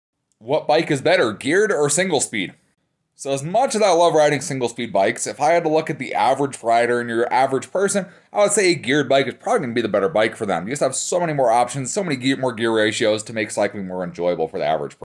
What bike is better, geared or single speed?
When it comes down to bikes there's a lot of options out there. What bike is better, geared or single speed? Let's talk about it!

1050 N Hills Blvd #60554